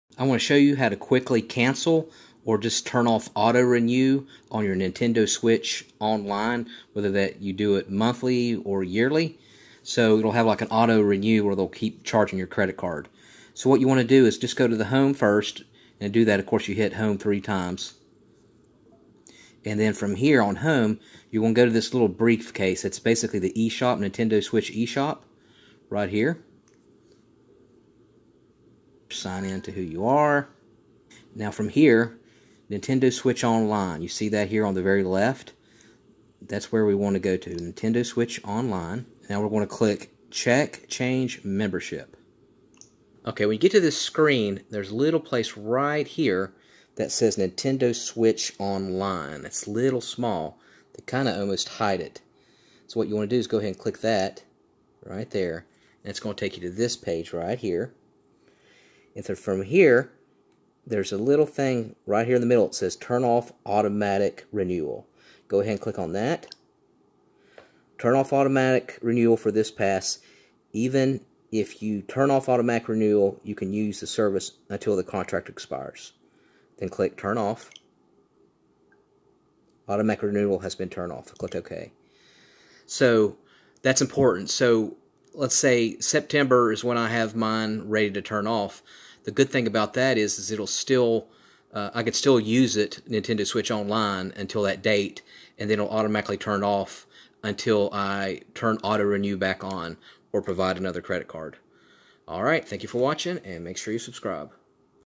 How TURN OFF Auto Renew Pay NINTENDO SWITCH ONLINE Console (eShop Monthly Yearly Credit Card Charge
To turn off auto-renewal STOP (automatic renewal pay for Nintendo Switch Online, you can follow these steps:
Start by accessing the Nintendo eShop on your Nintendo Switch console.
Ensure that you are signed in with the Nintendo Account associated with your Nintendo Switch Online subscription.
Once you are in the eShop, scroll to the bottom of the screen and select the user profile icon, which is usually located on the right side.
From the options presented, choose "Settings/Other."
In the "Settings/Other" menu, select "Nintendo Switch Online."
Look for the "Automatic Renewal" option and select it.
A confirmation message will appear. Choose "Turn Off" or a similar option to disable automatic renewal. nintendoswitch STOP AUTO PAY!

By completing these steps, you should have successfully turned off the auto-renewal for your Nintendo Switch Online subscription. Keep in mind that you will continue to have access to the subscription until the current period ends, after which it will not renew automatically.